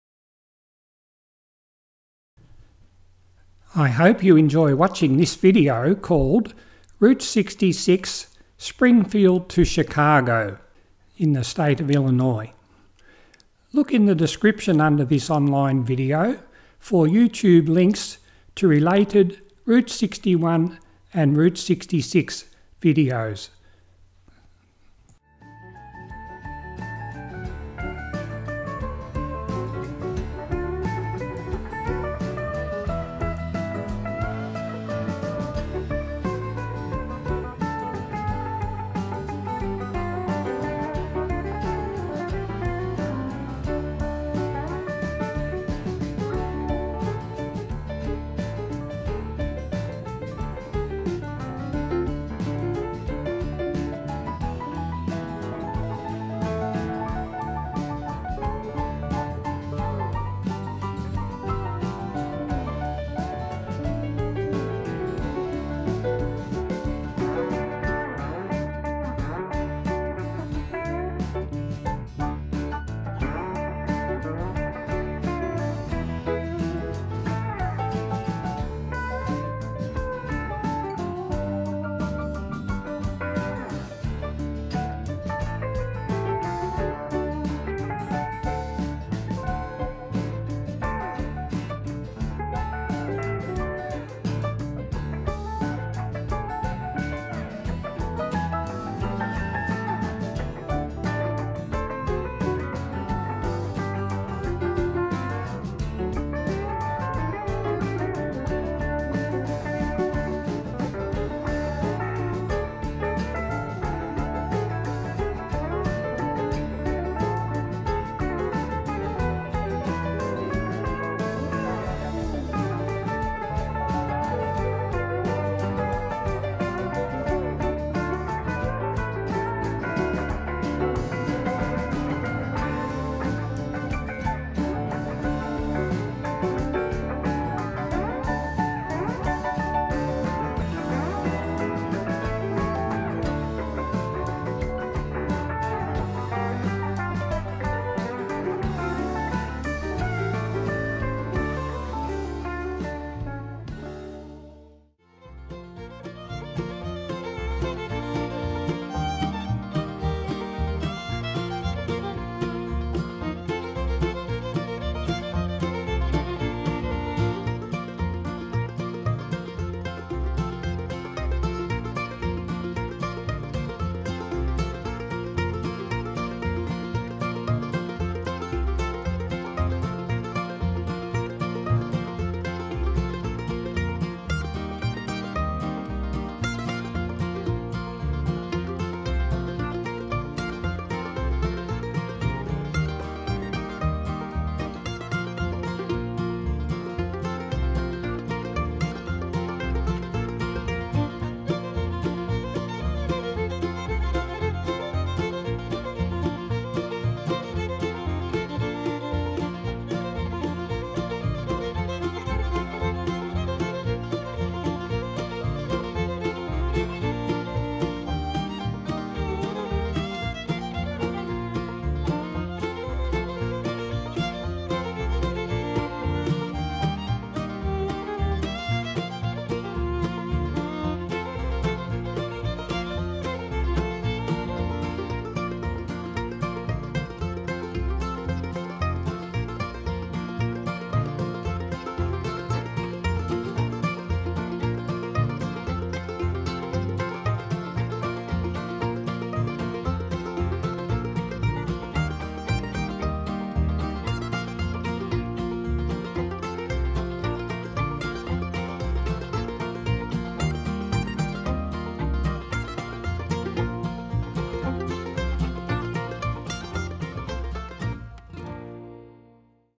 Route 66 Springfield to Chicago
Route 66 Springfield to Chicago, is part of a road trip from New Orleans in Louisianna along Route 61 and Route 66 up to Chicago in Illinois. YouTube links to related videos are shown below in date order and being 1080p FHD they are best viewed on a large computer screen.